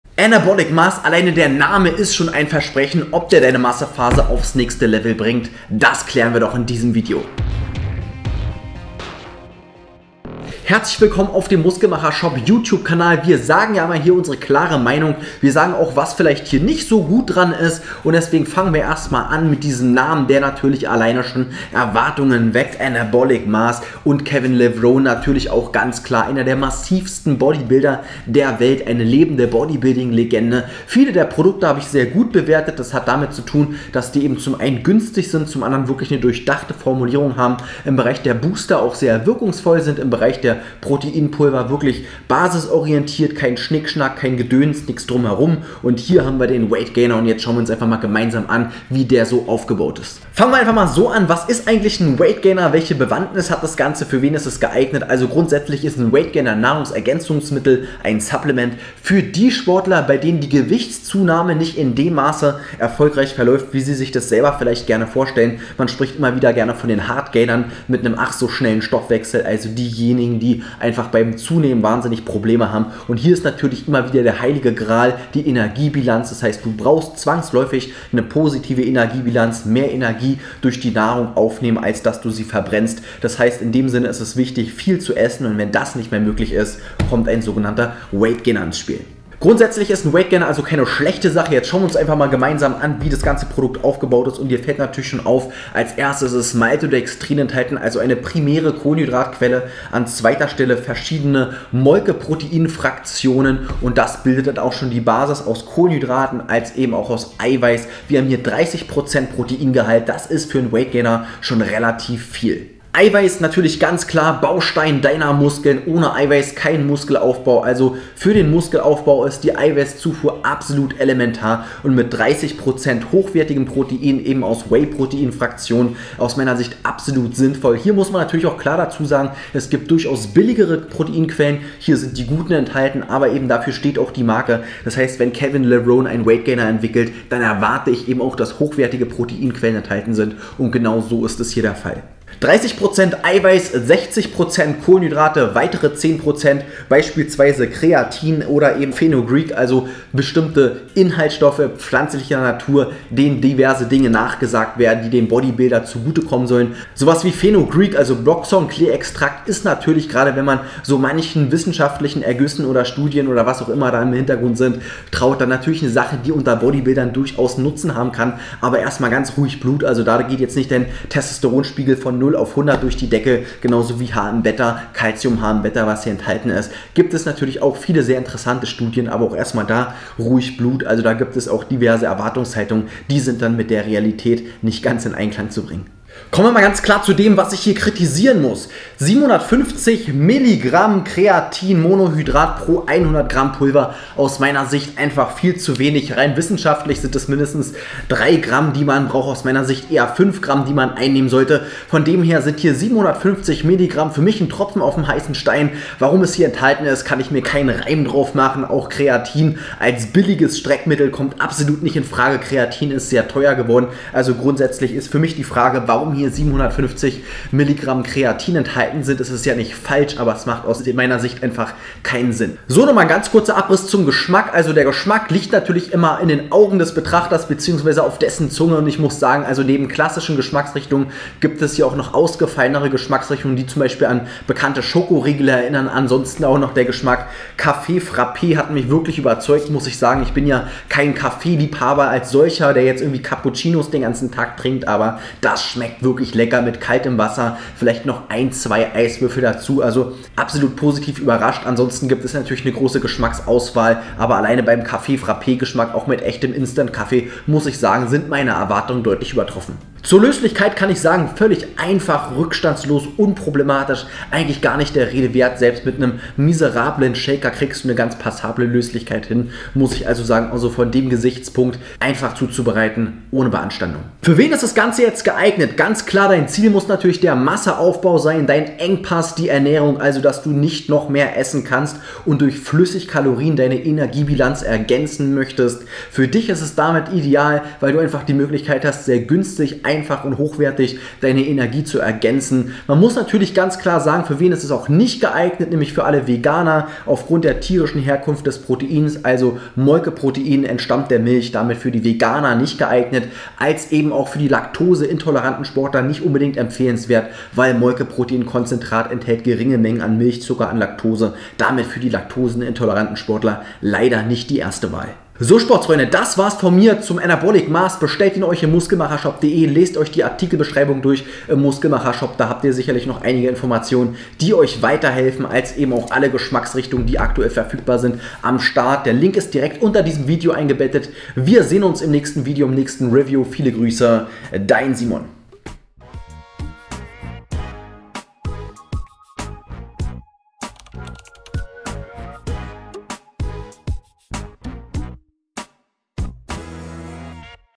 Kevin Levrone Anabolic Mass 💪 Weight Gainer im Test ✅ Muskelmacher Shop Review
Der Anabolic Mass von Kevin Levrone ist ein Supplement für den Masseaufbau und die Massephase. Der Weight Gainer überzeugt mit 30% Protein und nützlichen Kohlenhydraten. Außerdem enthält der Kevin Levrone Anabolic Mass weitere Inhaltsstoffe wie Creatin und eine "Testo-Booster-Matrix". In diesem Video erklärt dir Simon alle Vor- und Nachteile des Kevin Levrone Anabolic Mass